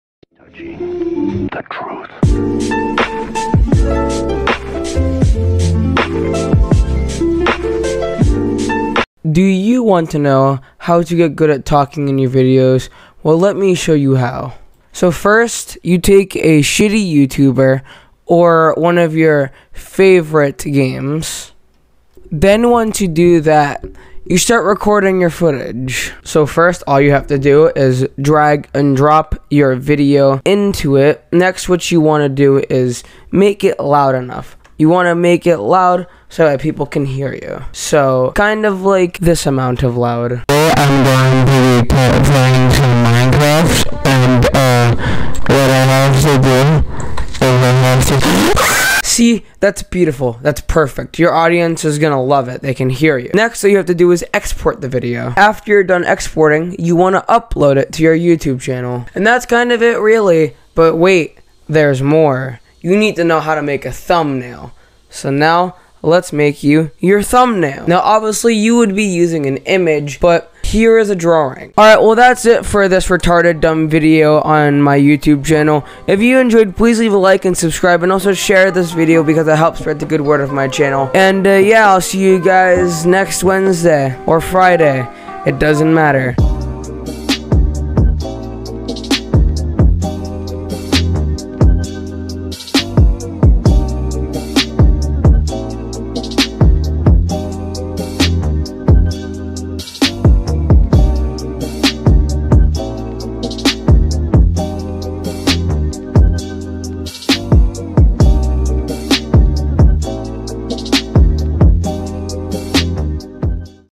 How to edit and get good at commentary
Hello and are u looking for help on how to be good at editing and commentary not really well you have come to the right video also sry about the croping of this video